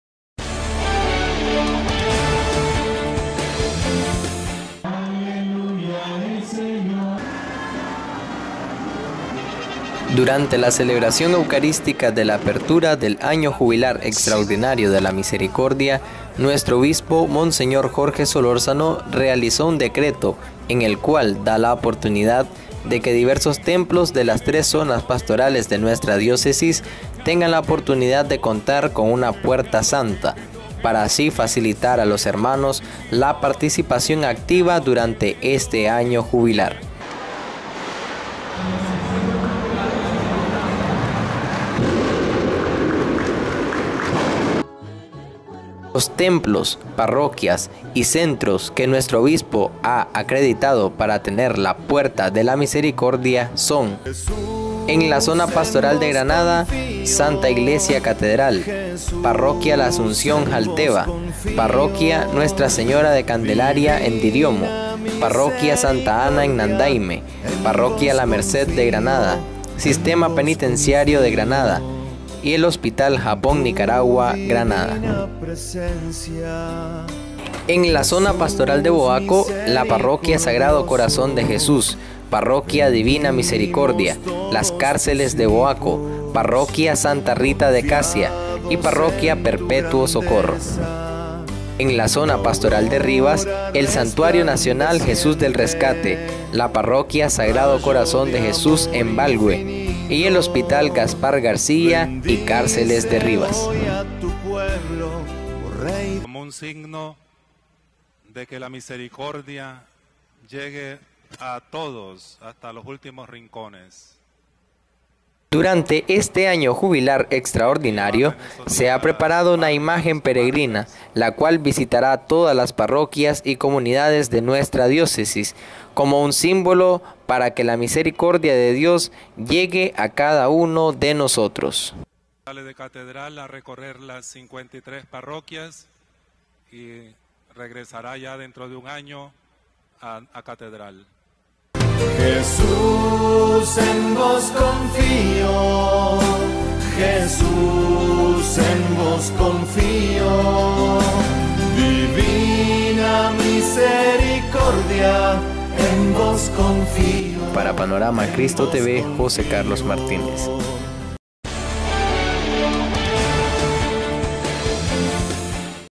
Templos y parroquias acreditados para la Puerta de la Misericordia
Monseñor Jorge Solórzano decreta que templos, parroquias y centros de las tres zonas pastorales de la Diócesis cuenten con la puerta santa para facilitar la participación activa de la población durante el año jubilar de la misericordia